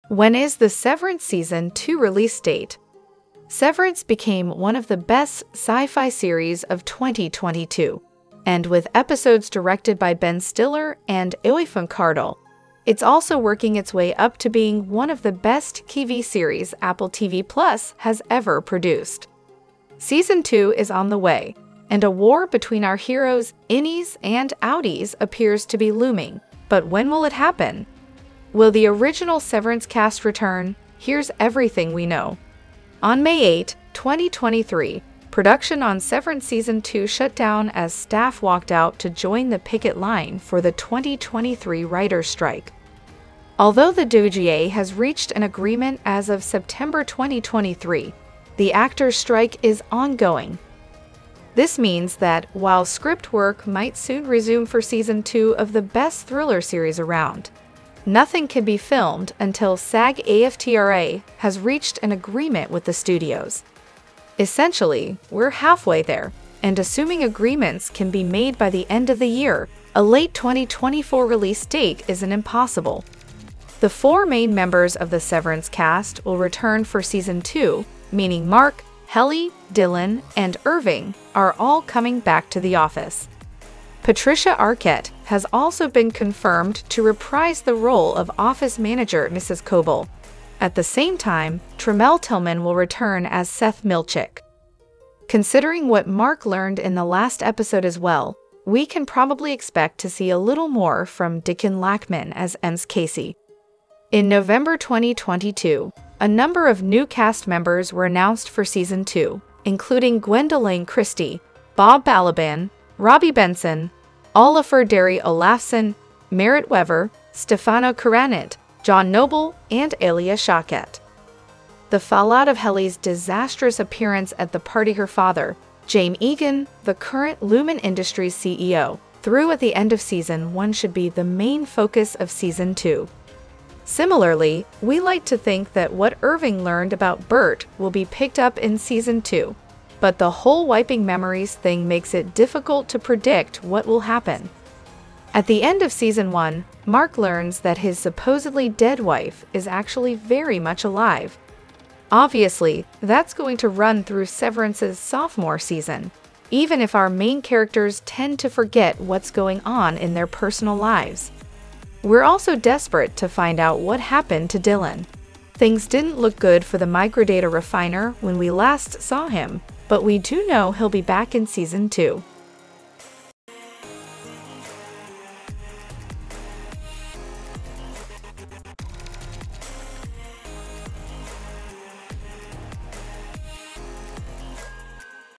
SEVERANCE SEASON 2 release date, cast, plot and more news
OUR OTHER YOUTUBE CHANNEL-  @NamiNohara

Call of Duty player blacklisted for her bikini pics Severance became one of the best sci-fi series of 2022, and with episodes directed by Ben Stiller and Aoife McArdle, it’s also working its way up to being one of the best TV series Apple TV Plus has ever produced.

📺 MOVIES 🎟️ WEB SERIES 🍿 ANIME & MORE 🎥

✍️Written by:  SOFIA ROSE
🎙️Narrated by: MIA DI ANDRAD
🧑‍💻Edited by: KEVIN REY